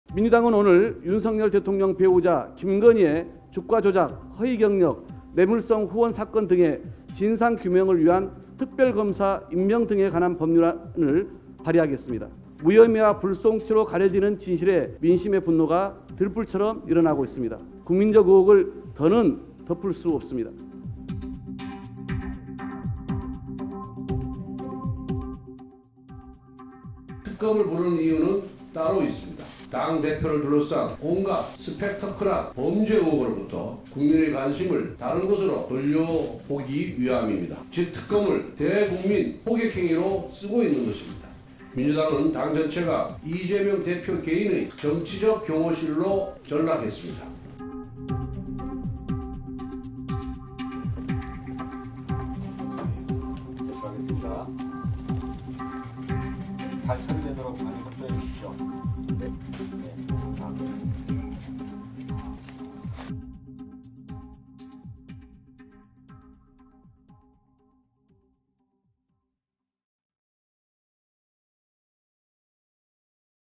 이재명 대표, 김건희 여사 둘러싼 여야의 대격돌｜D:이슈
민주당이 이재명 대표의 기소를 앞두고 '김건희 특검법'을 발의하기로 했습니다. 이에 국민의힘은 즉각 반발했는데요. 추석을 앞두고 정국이 급속 냉각되고 있습니다.